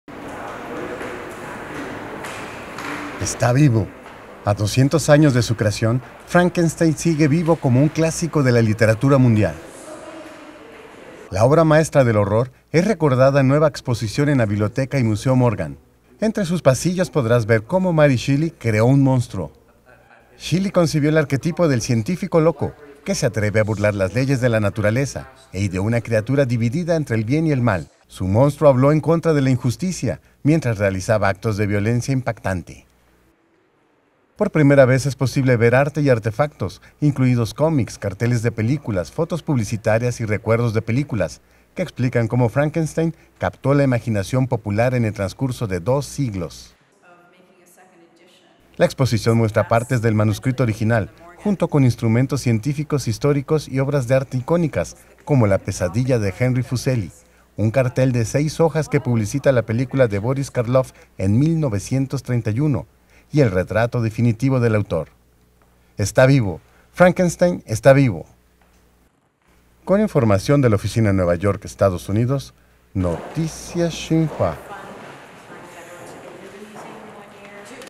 ‘¡Está vivo!, 200 aniversario del Frankenstein de Mary Shelley
Frankenstein, la obra maestra del horror, es recordada en nueva exposición en la Biblioteca y Museo Morgan, a dos siglos de su creación.